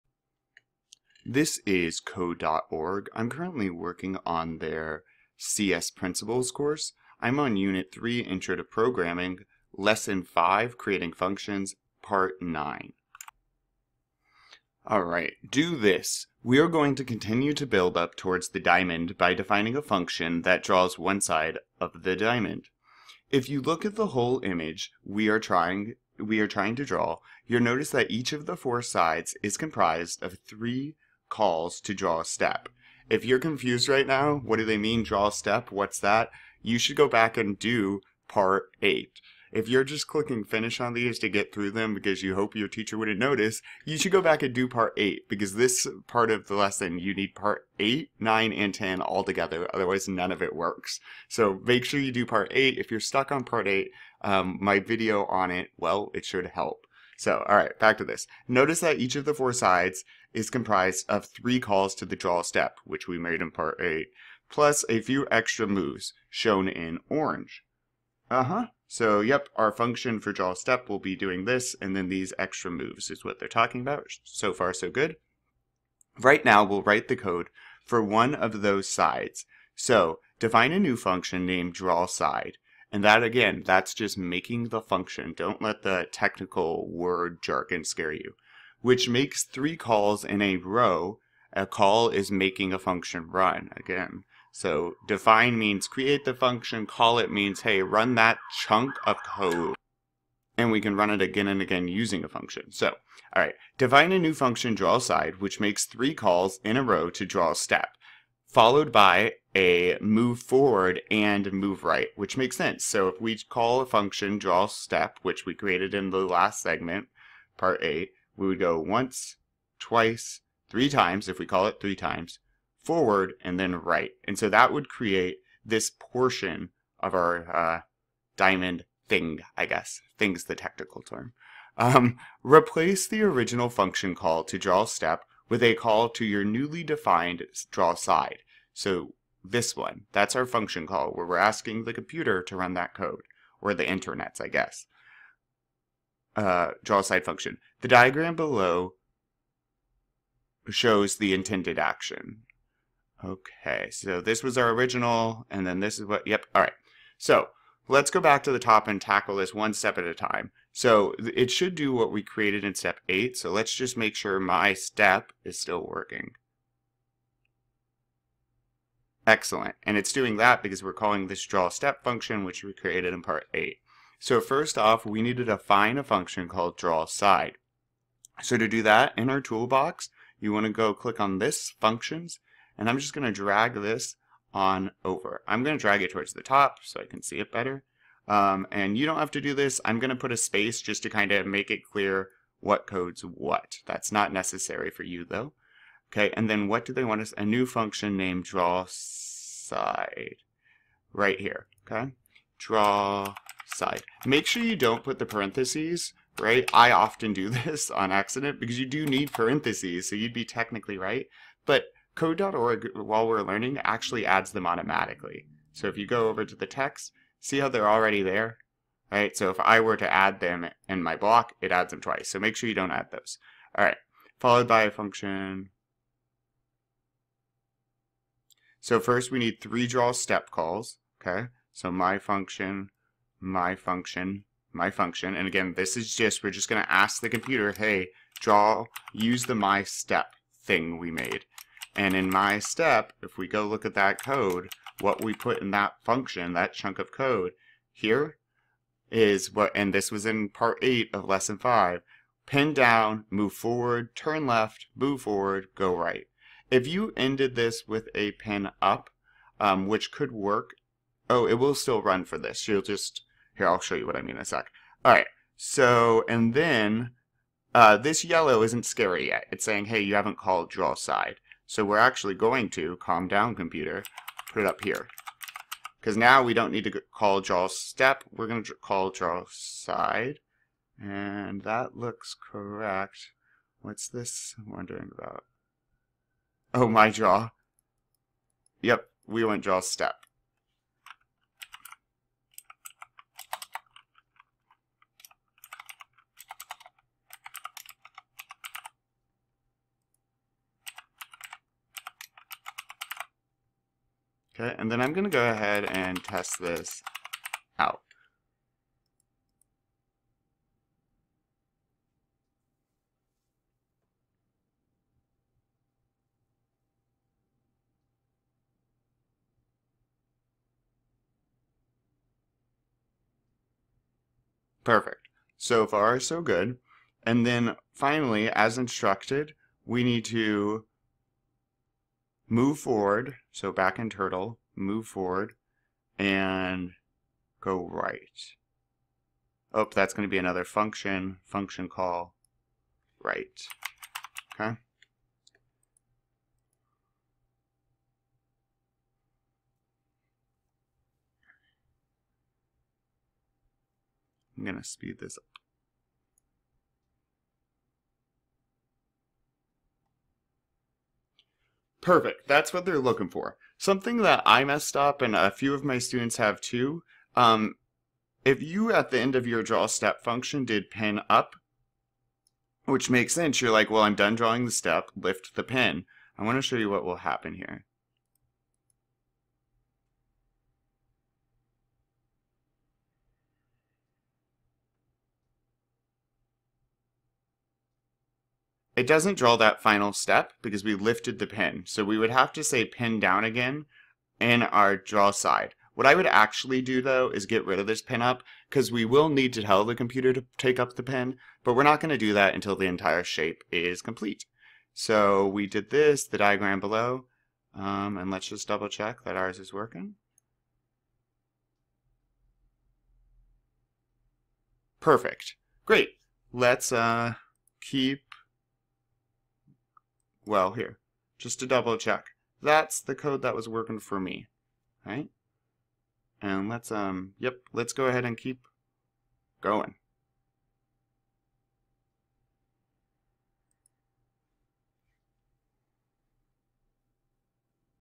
Creating Functions Lesson 5.9 Tutorial with Answers Code.org CS Principles
Creating Functions, lesson 5, is from Building Apps, unit 5 of Code.org's C.S. Principles course, often used in AP Computer Science classrooms. Each part of the course challenges students to accomplish a new task while learning principles of computer science. In this video I walk through step-by-step how to get the right answer for this puzzle. Tutorials for other lessons is linked below.